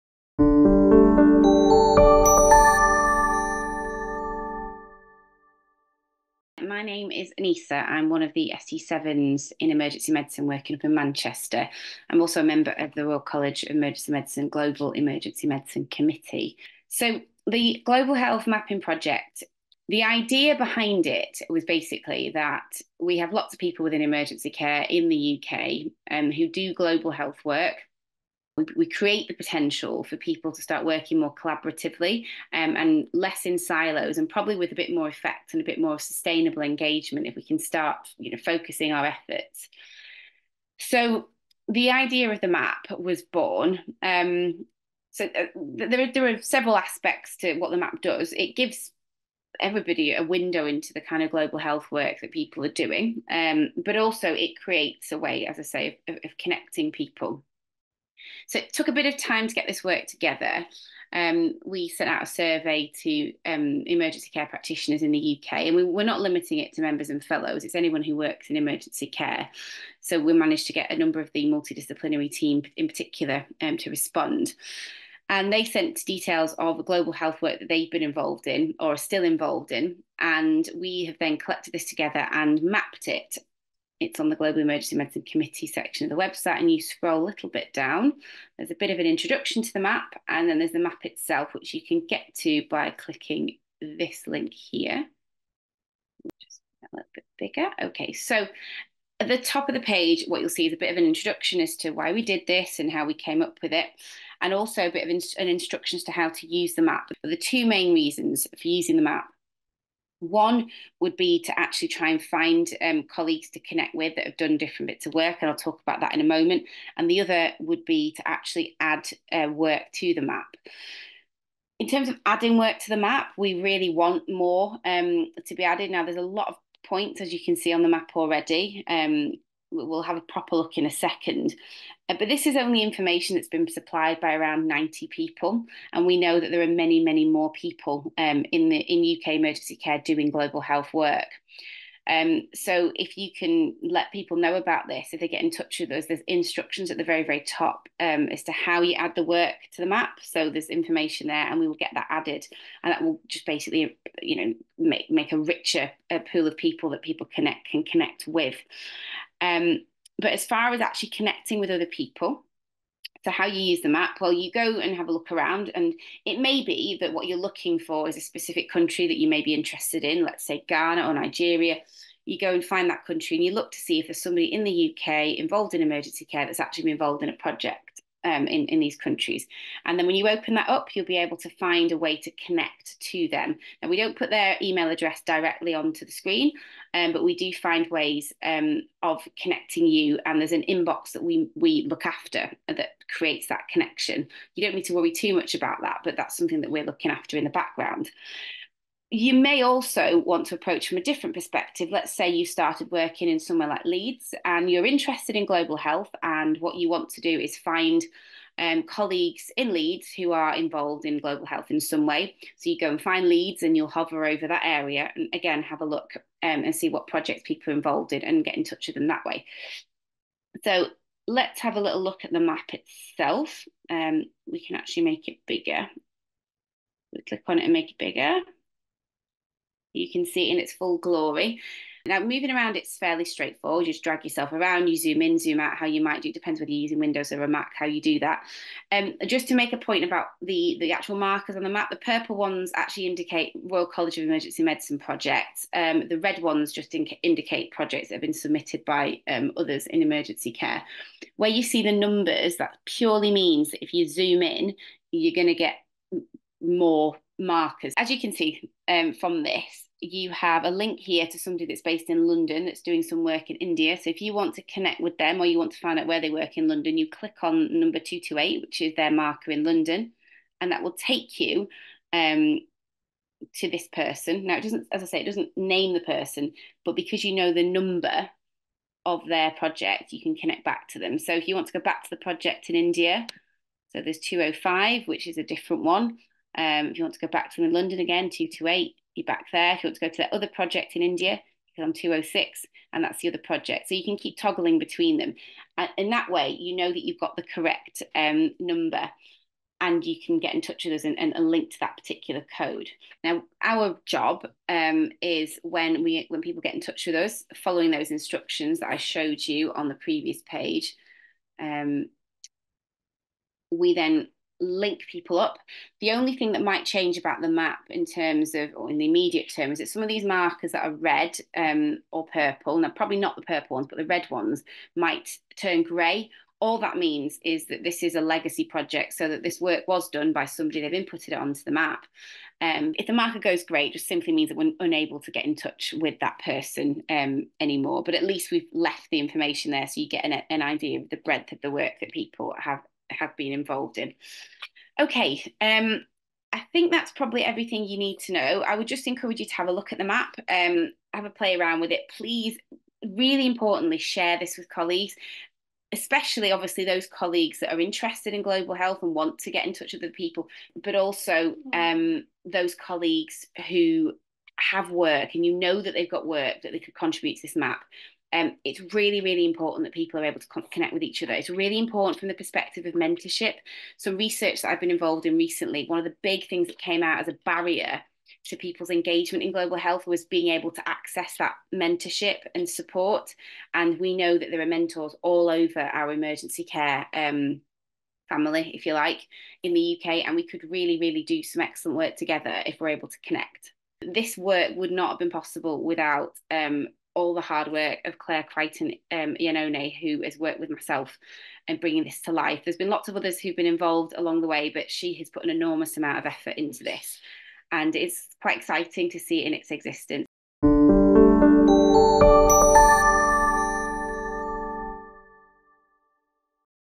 Get involved in a global health mapping project
Anisa Jafar from RCEM's Global Emergency Medicine (GEM) Committee explains how an interactive global health map is connecting projects with clinicians.

The map, led by the work of the RCEM Global Emergency Medicine (GEM) Committee, is improving the quality and sustainability of projects by sharing them with a global network of medical clinicians and inviting more to contribute.

Anisa Jafar and Claire Crichton-Iannone launched the project in 2021 with the RCEM GEM Committee and a team of colleagues and the University of Manchester.